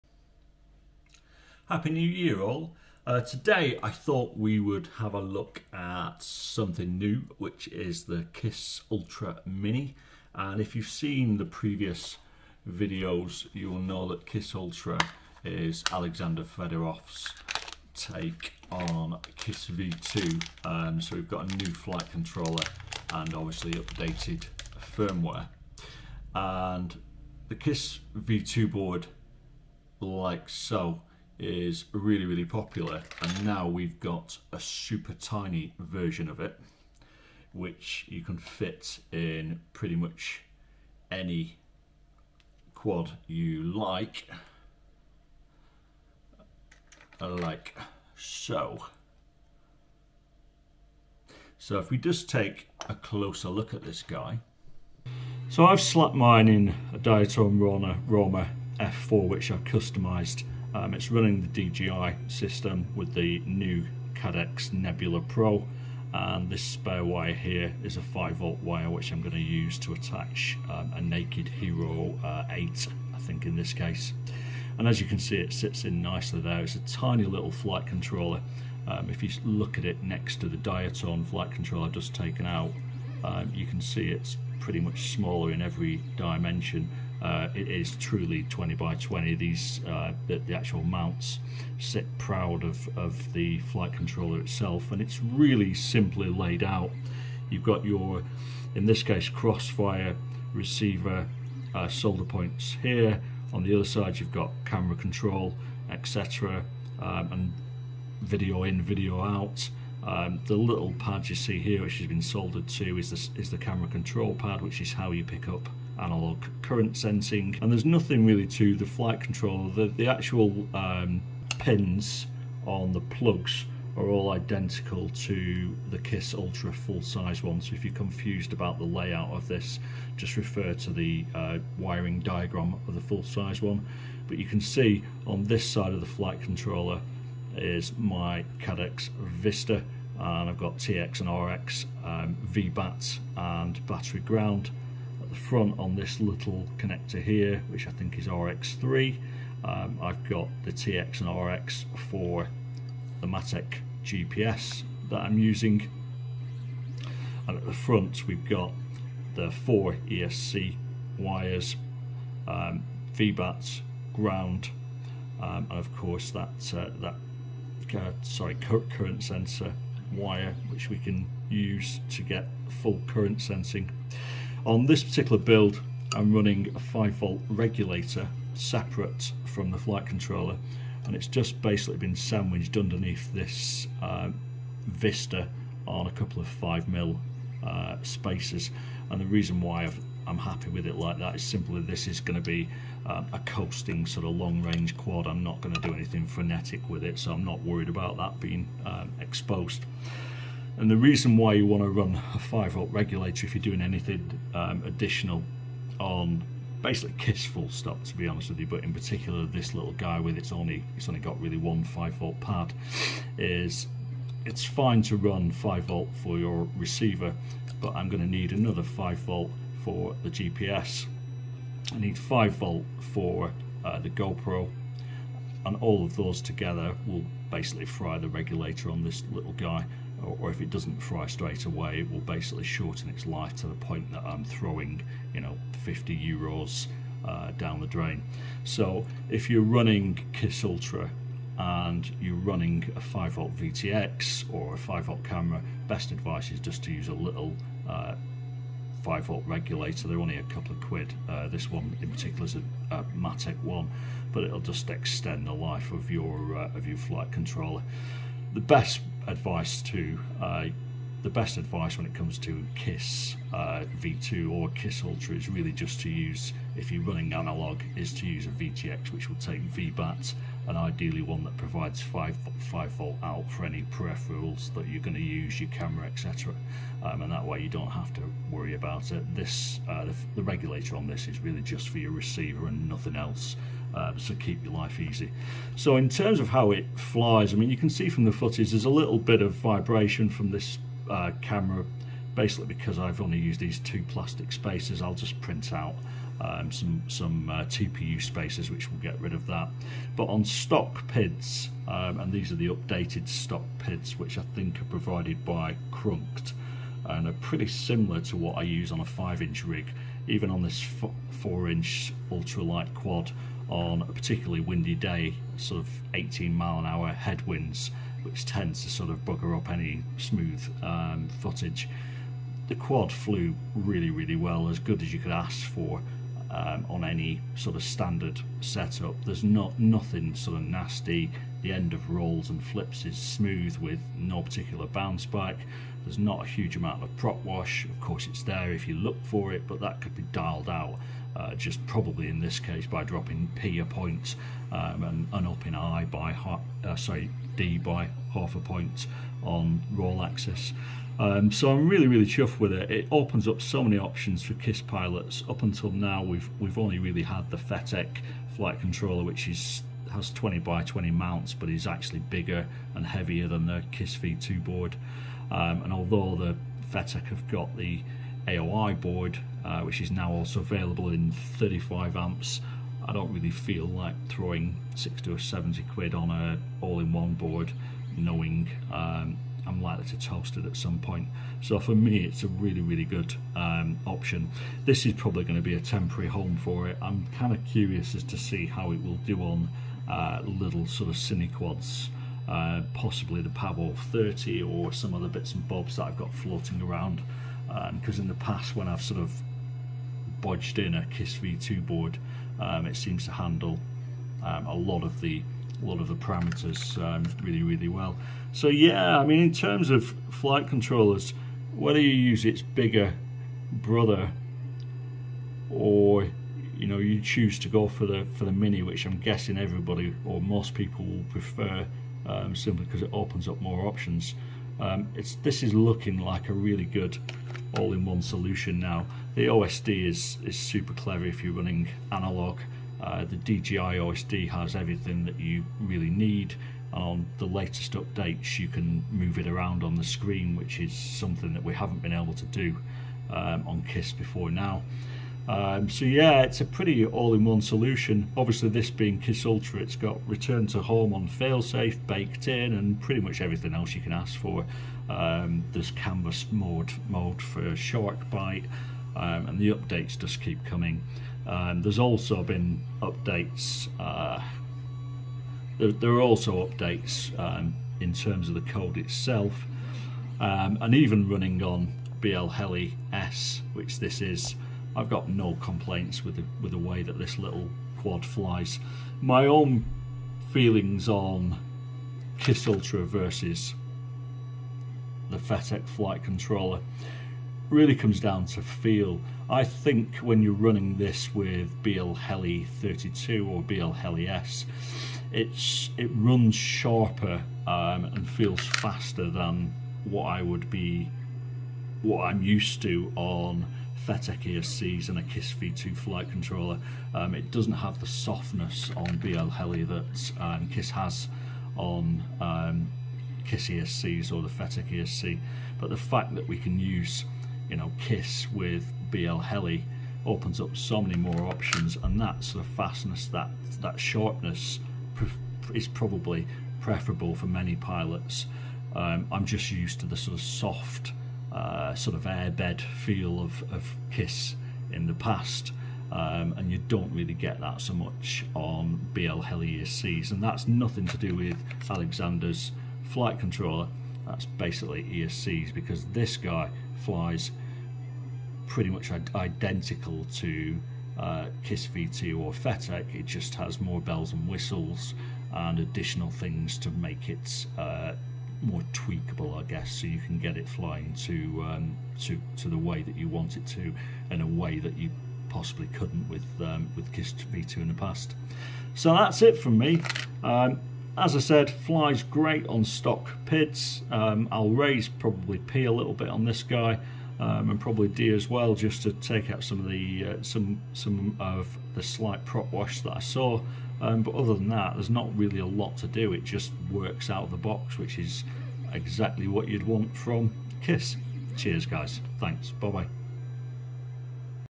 Kiss Ultra Mini 20x20
Look and flight of the new Kiss Ultra 20x20 flight controller.